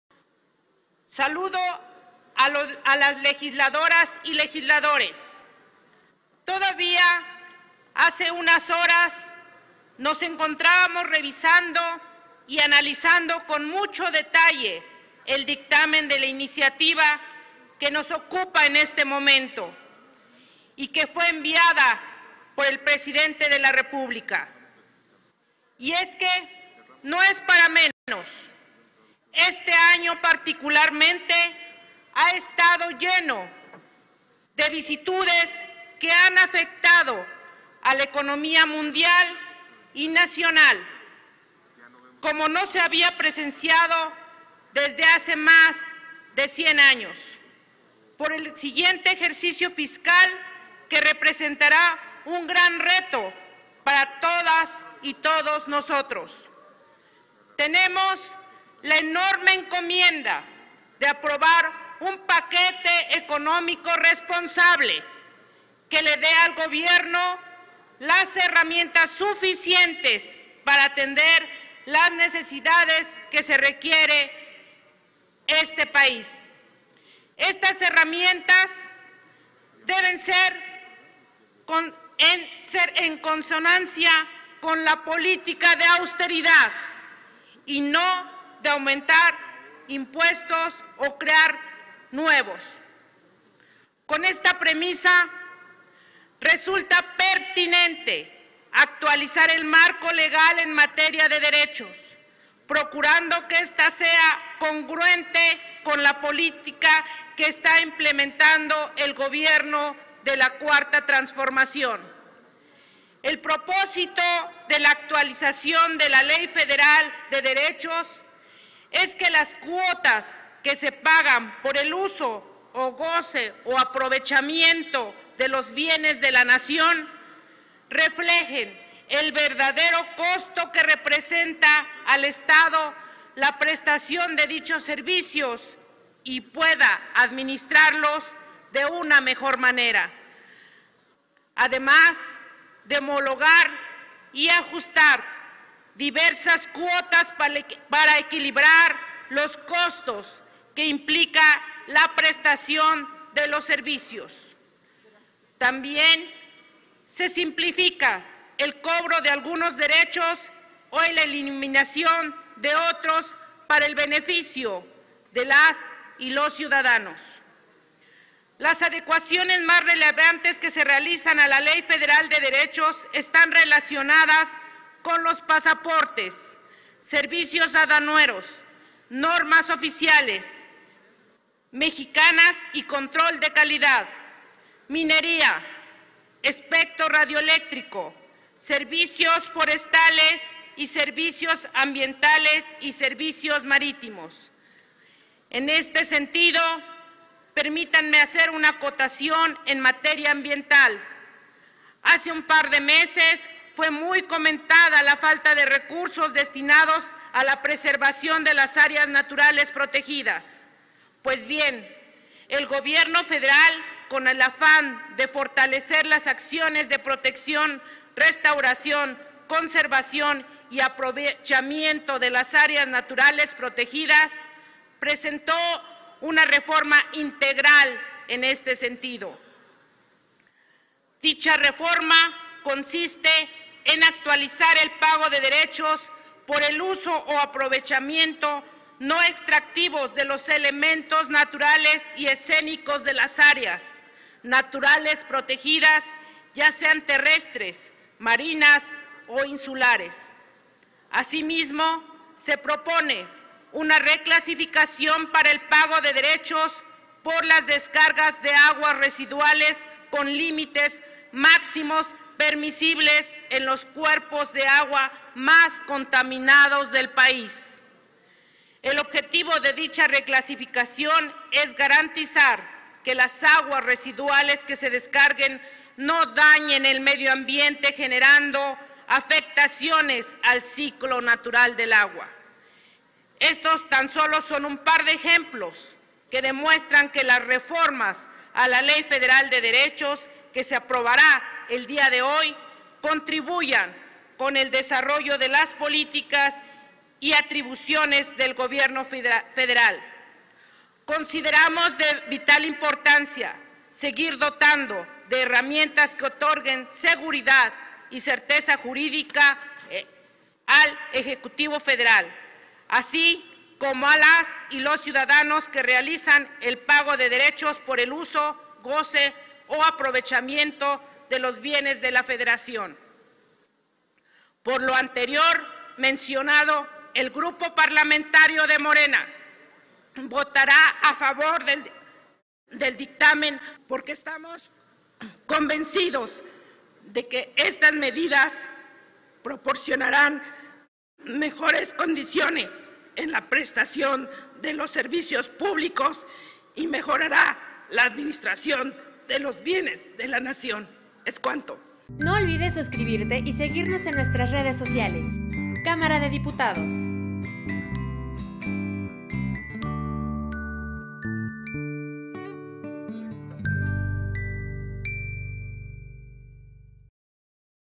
Dip. Alejandra Pani Barragán (Morena) / Dictamen sobre la Ley Federal de Derechos.
La diputada Alejandra Pani Barragán (Morena) presenta posicionamiento sobre el dictamen que reforma, adiciona y deroga diversas disposiciones de la Ley Federal de Derechos.